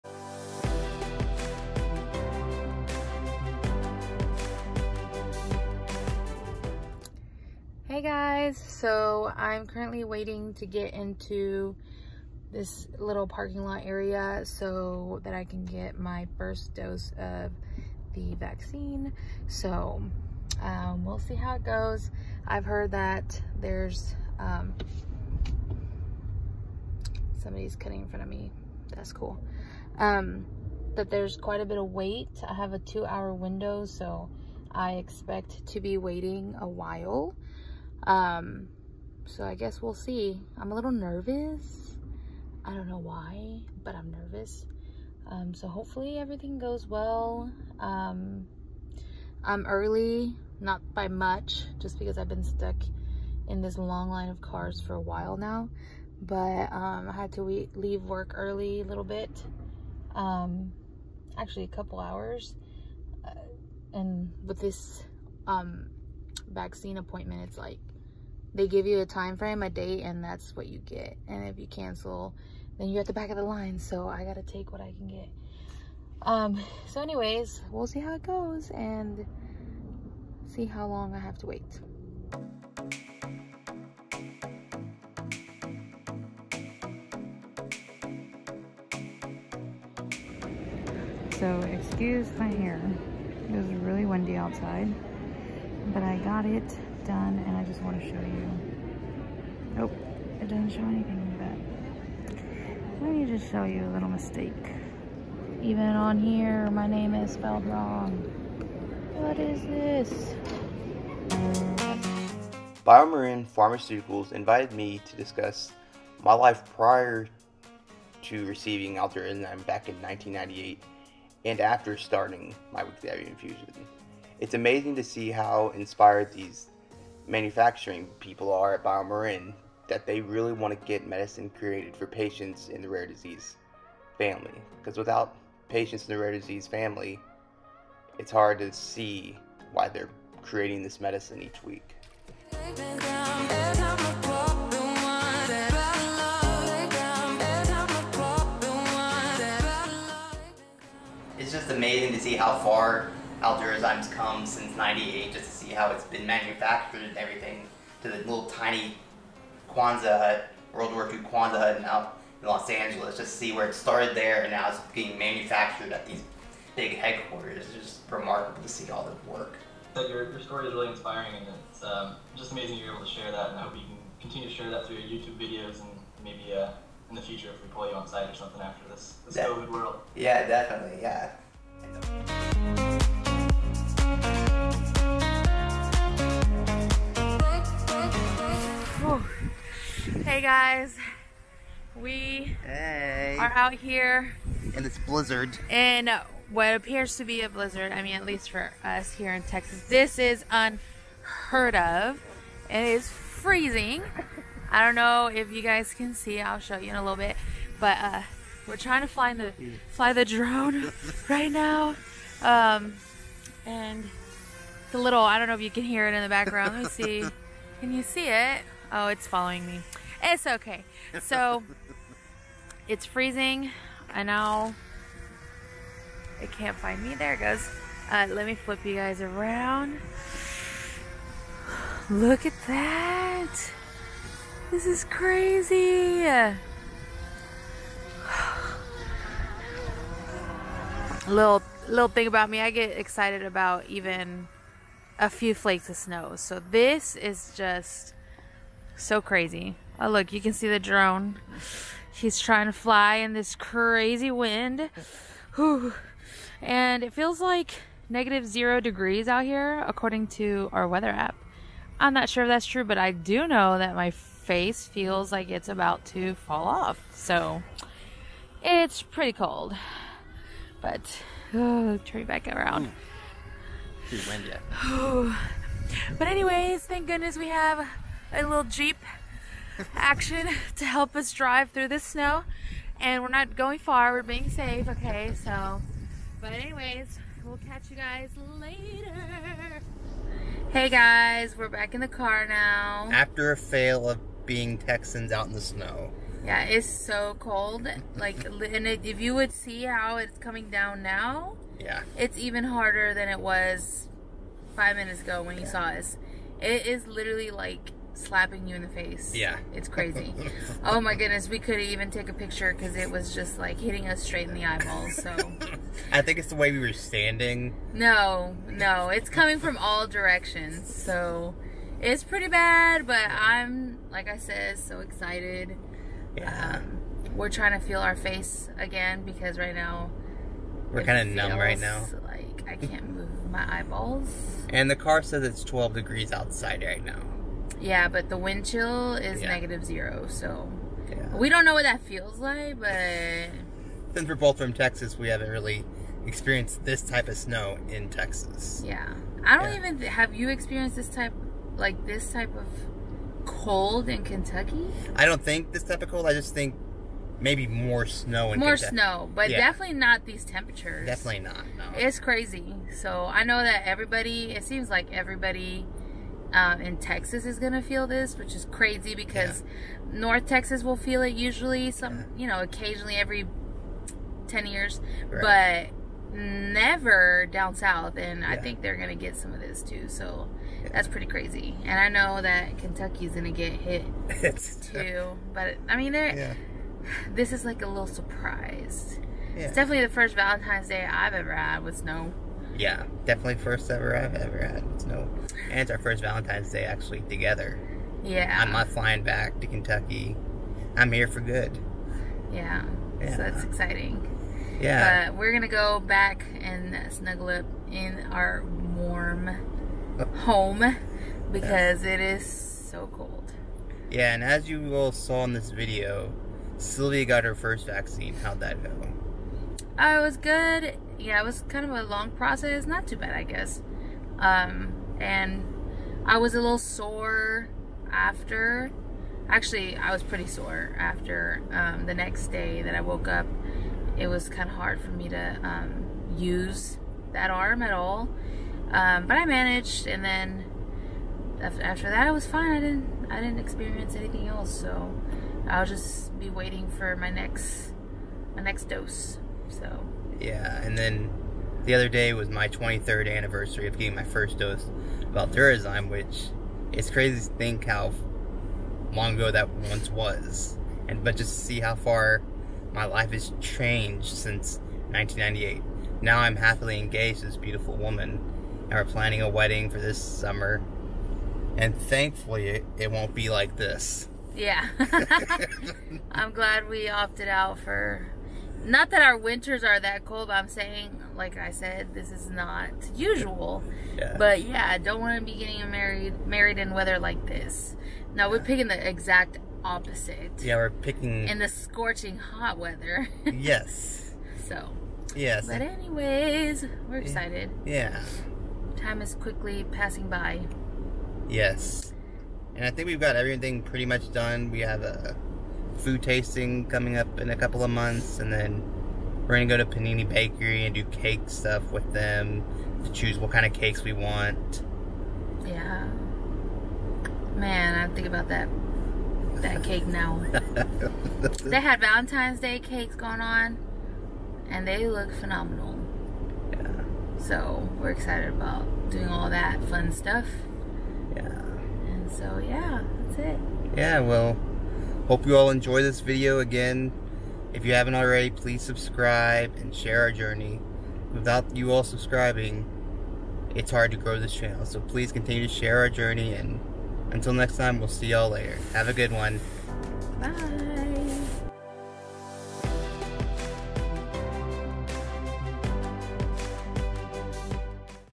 A Rare Journey: Texas Blizzard
Silvia gets her first Covid19 Vaccine, Ryan talks to the BioMarin Pharmaceutical Inc. manufacturing team, and a Texas blizzard. @Rarities